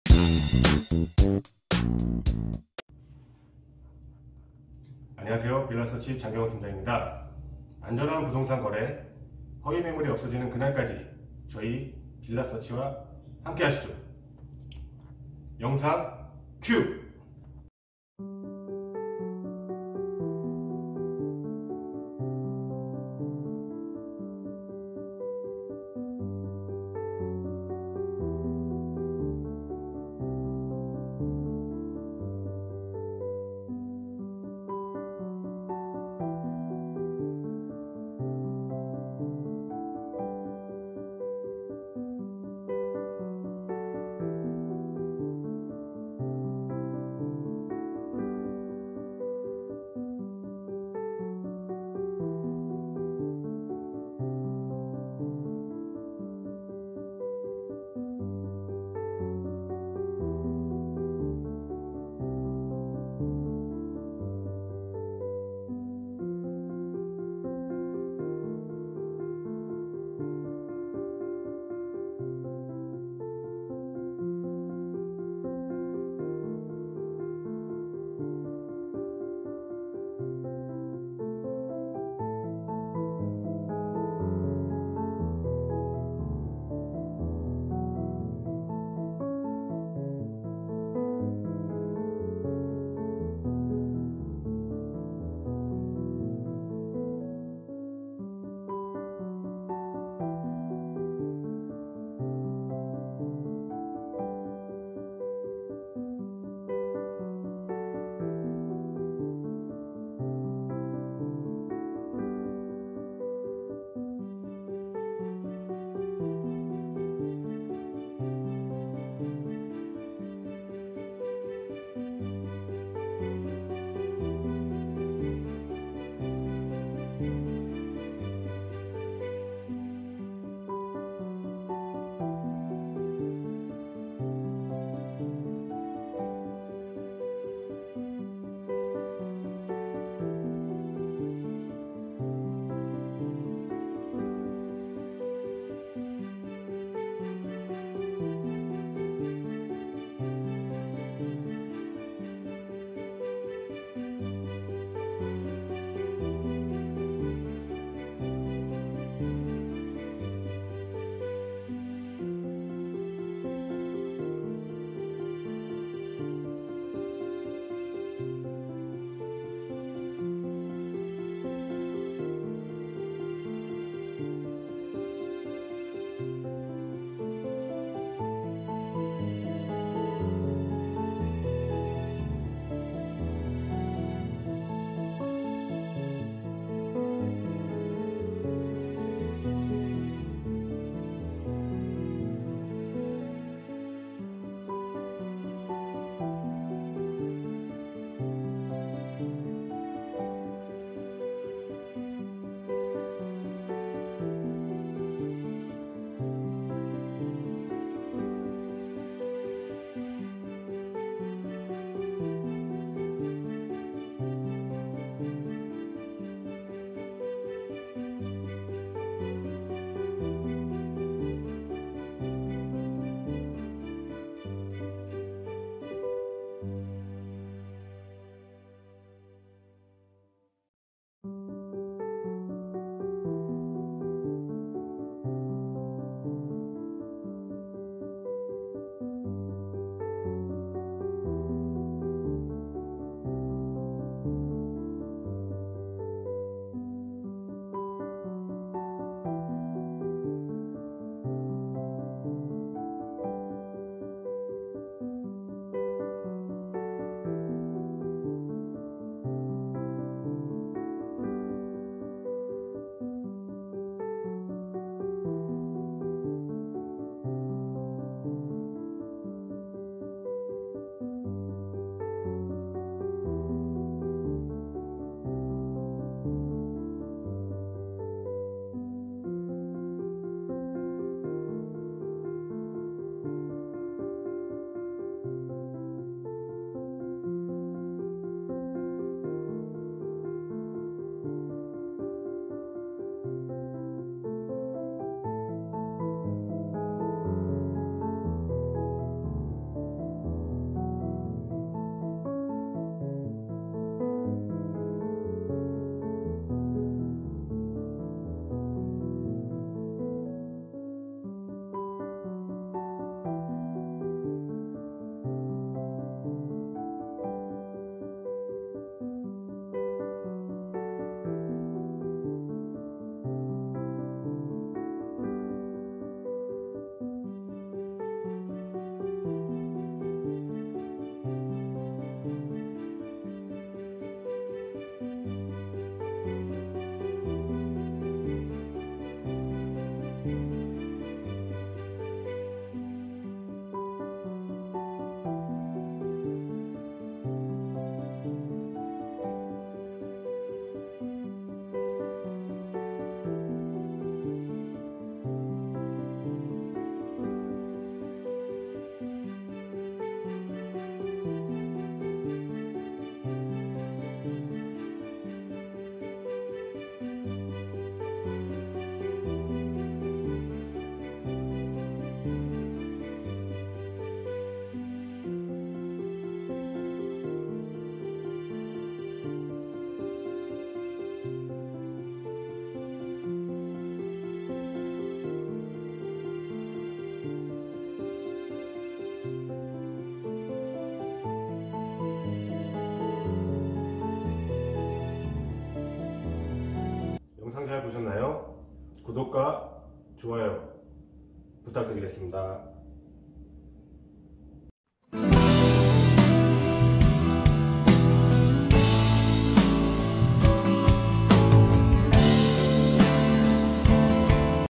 [파주신축빌라]야당동 느낌있는 복층을 원한다면 바로여기!!
✔️위치: 파주시 야당동
✔️빌라명: 스페이스카운티
✔️실평수: 48평 복층
✔️실입주금: 최저입주금 및 최대융자가능(유선안내)

365일 연중무휴(친절상담)